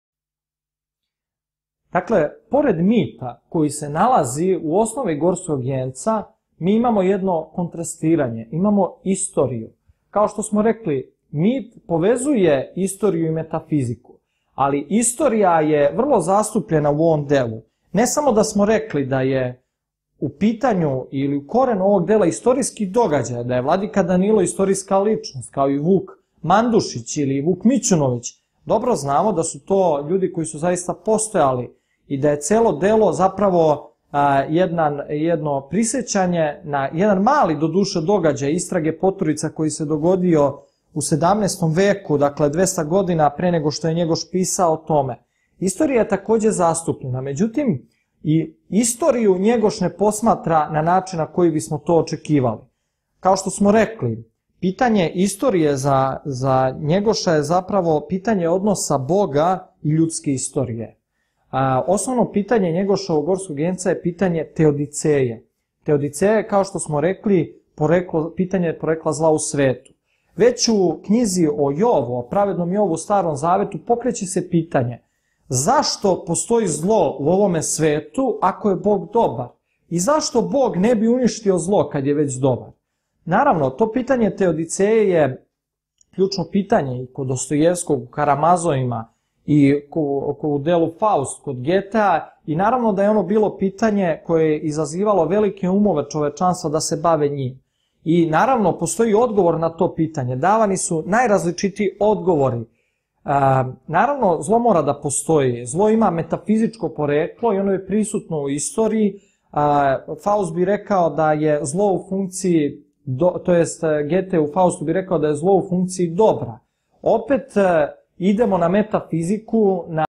Petar Petrović Njegoš - Gorski vijenac (šesti deo) - srpski jezik II
Sukob mita i istorije u ovom delu. Istorija u Gorskom vijencu. Da li je vreme kružno ili linearno? Istraga poturica je pitanje o Bogu. Da li istorija vodi ponavljanju ili razrešenju? Istraga poturica zbog toga postaje metafizičko pitanje. Vojvoda Draško - sukob tri civilizacije: Evropa, Turska, Crna Gora.
Kompletan video možete pogledati na